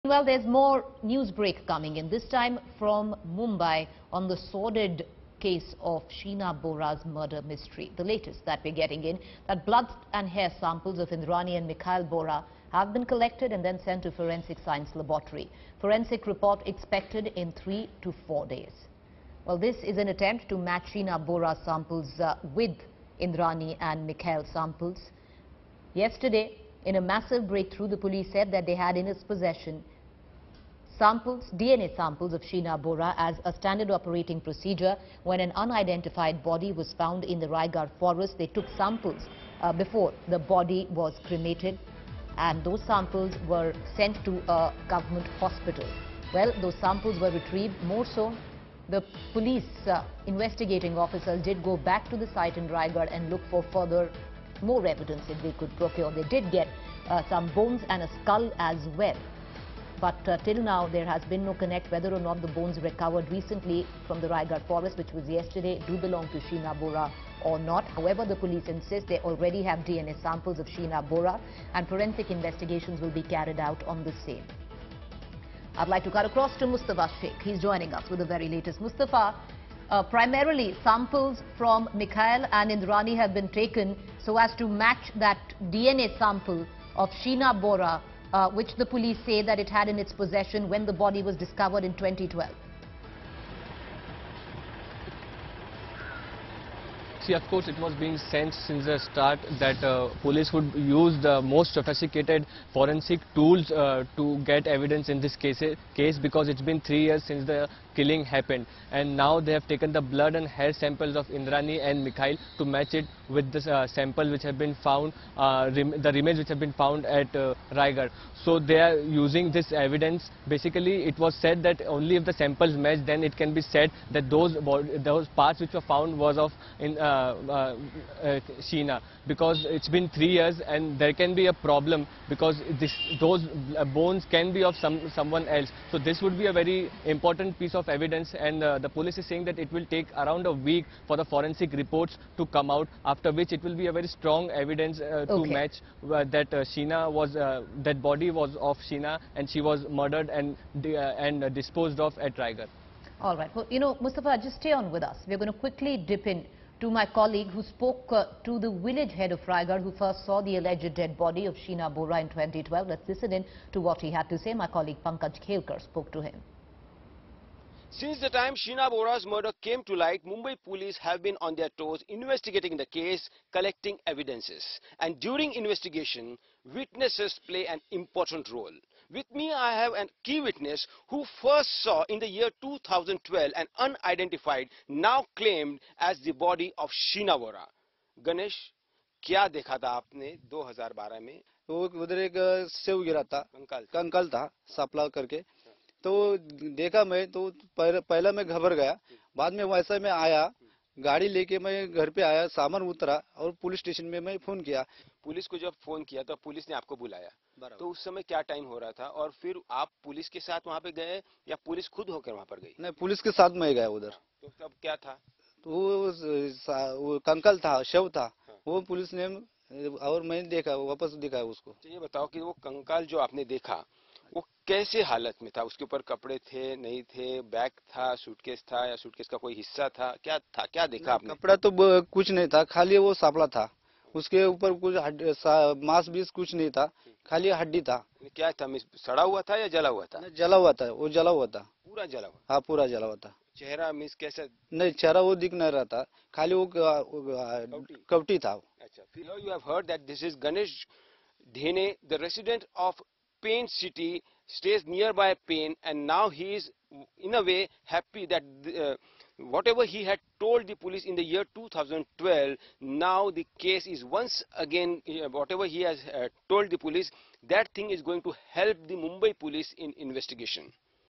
Sheena Bora case: Indrani, Mikhail's DNA samples sent to forensics
Blood and hair samples of Indrani Mukerjea and Mikhail Bora have been sent to the forensics for futher investigation.